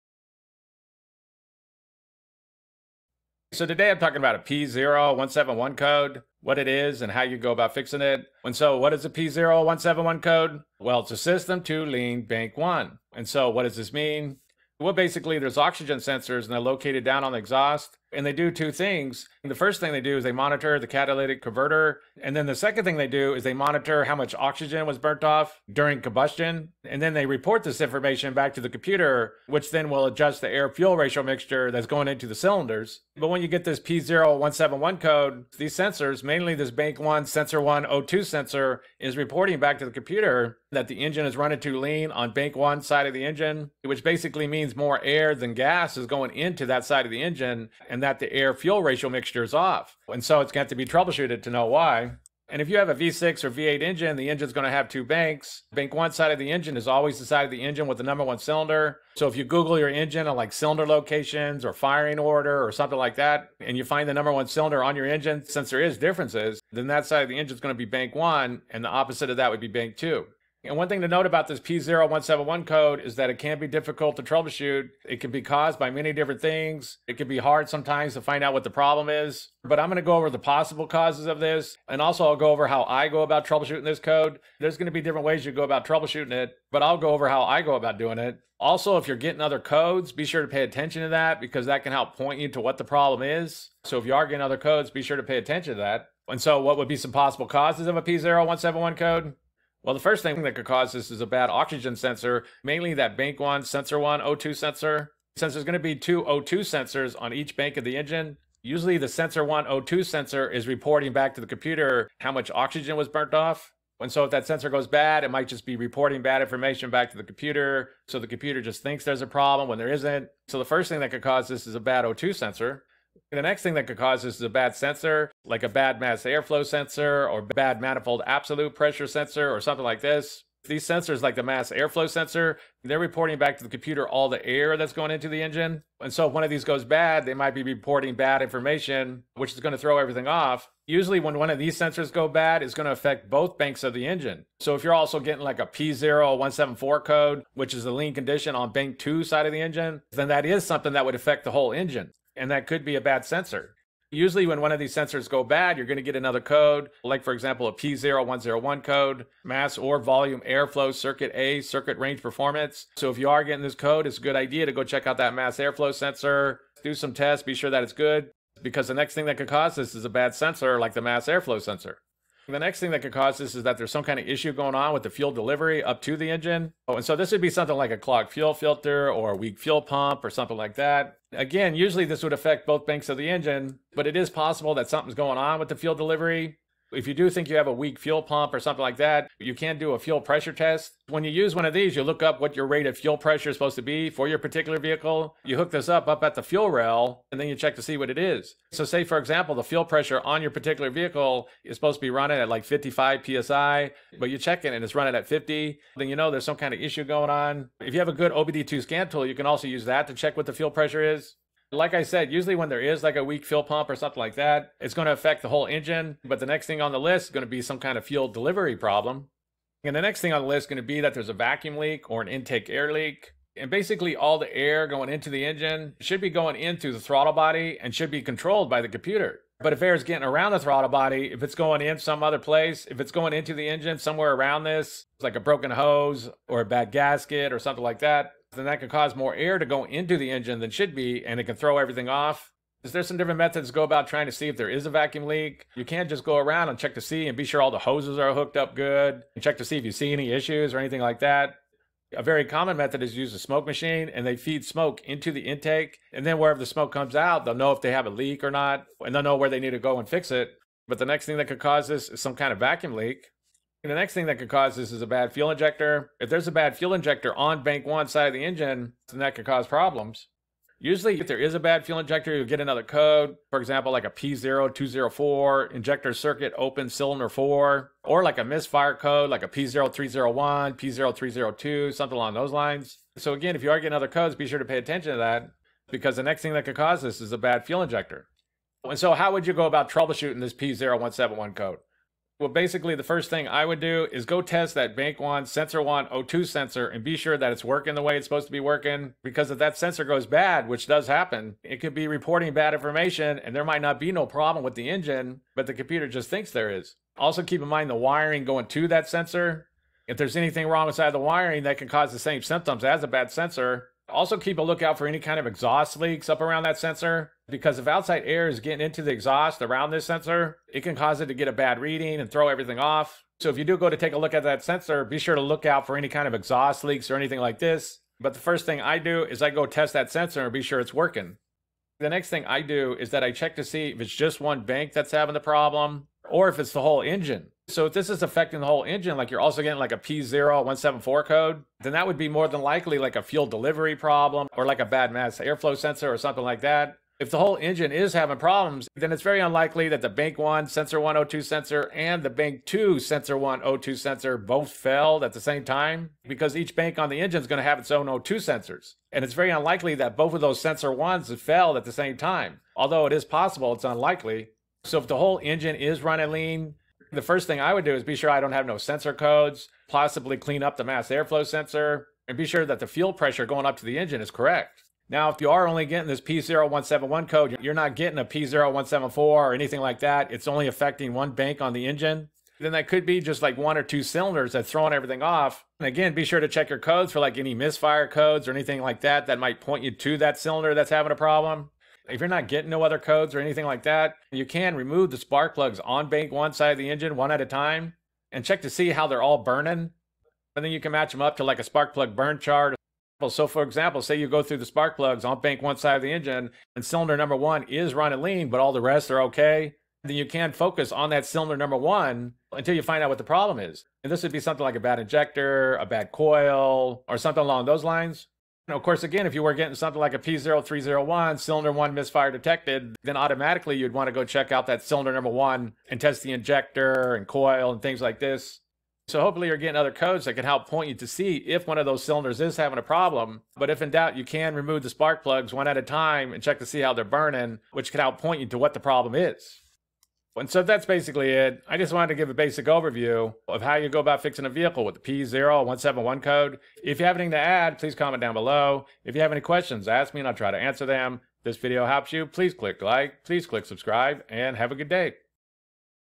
Troubleshooting P0171 Code: System Too Lean (Bank 1)
How I diagnose and troubleshoot an engine P0171 Code: System Too Lean (Bank 1)
A lean condition exists if the engine is getting more air than fuel in the cylinders. (Air/Fuel Ratio is off)
P0171 code is problematic code since it can be caused by several things.
If there are other codes pay attention to that since they can help point to the issue.
Possible Causes P0171 Code: System Too Lean (Bank 1):
Oxygen Sensor (Bank 1 Sensor 1)
Dirty Air Filter
Mass Air Flow Sensor (Codes P0100, P0101, P0102, P0103, P0104)
Fuel Delivery (Clogged Fuel Filter. Weak Fuel Pump)
Vacuum Leak
Bad Fuel Injector (Injector or Misfire Codes P030x P020x)

How I Troubleshoot a P0171 Code:
Test or Replace Bank-1 Sensor-1 Oxygen Sensor
If the 02 sensor is good, is the whole engine running lean or a single cylinder?
If there are no other codes, remove the spark plugs on Bank-1 and check how they are burning.

Is one cylinder or multiple cylinders having issues?
Multiple cylinders would point to something that would affect the whole engine, such as a bad sensor or fuel delivery problem.
A single cylinder misfire usually points to a problem on that cylinder, such as a bad injector, coil, or a wiring issue.